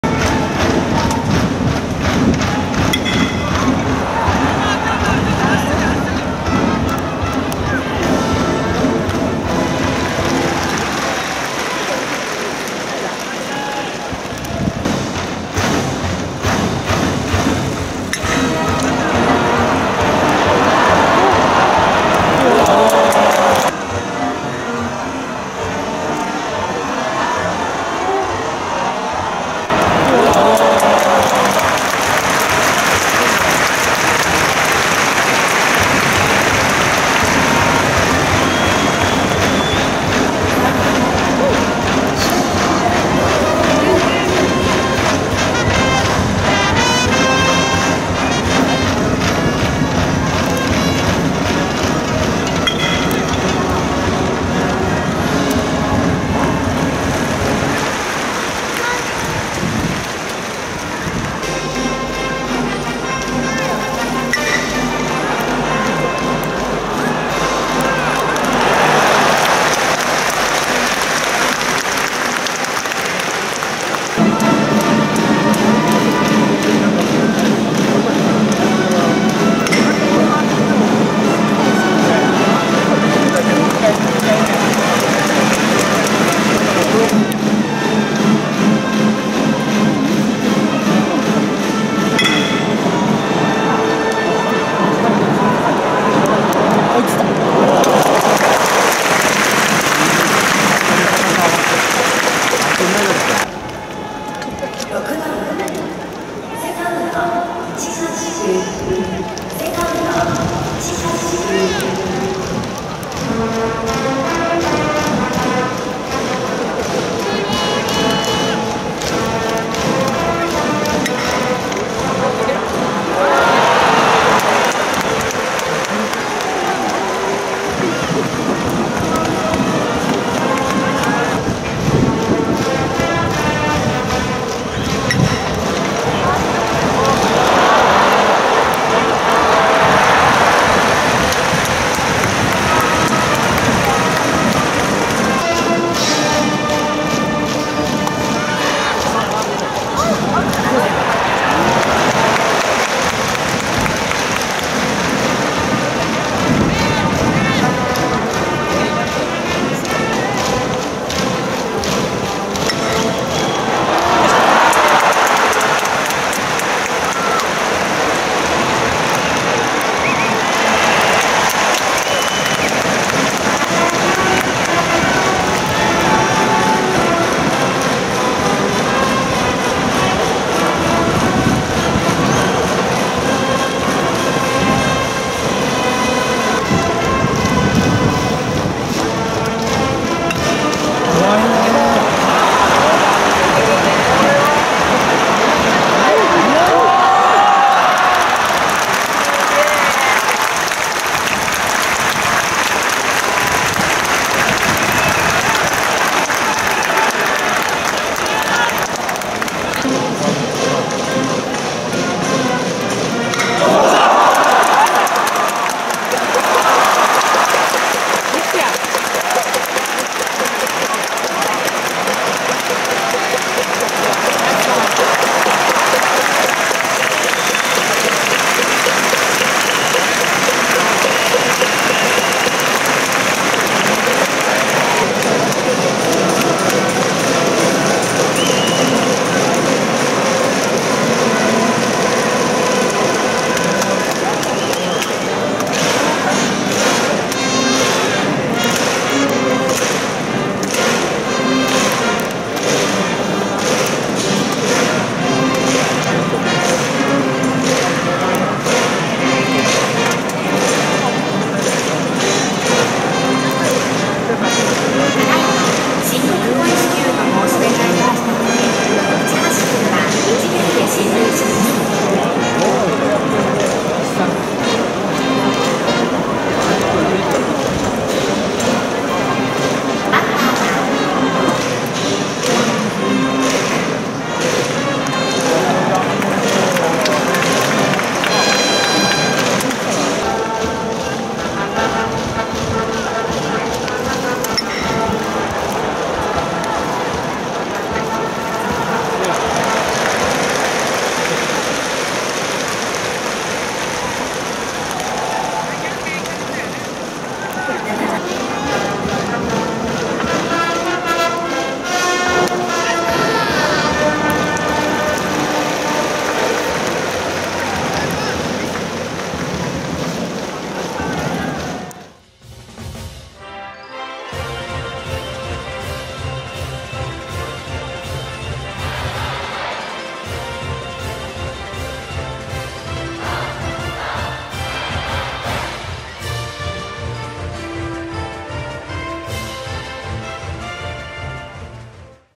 菊池涼介選手(広島)のような守備に場内がどよめく！50m5.8秒の超俊足で1塁到達タイムは3.71秒を記録！愛工大名電の韋駄天男 市橋昴士選手の守備＆打撃
第104回 全国高校野球選手権大会
愛工大名電の市橋昴士二塁手

身体能力の高さがよくわかる！
間違いなく愛工大名電のキープレーヤー！

※選手への誹謗中傷、過度な批判などのコメントは削除させてもらいます。